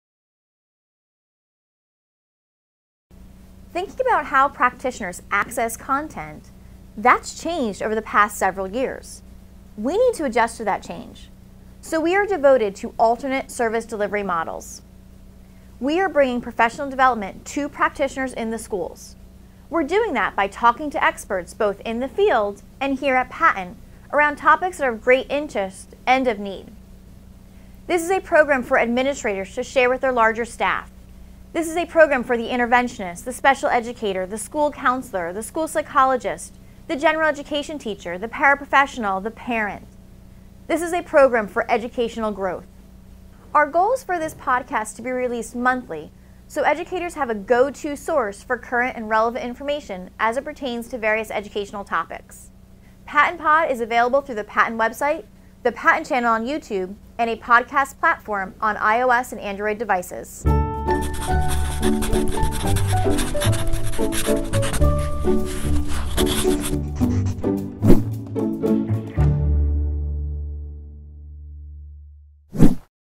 PaTTANpod Promo
Find out PaTTAN’s newest offering – PaTTANpod – a podcast series for educators

Also available in podcast format on iTunes and Google Play Music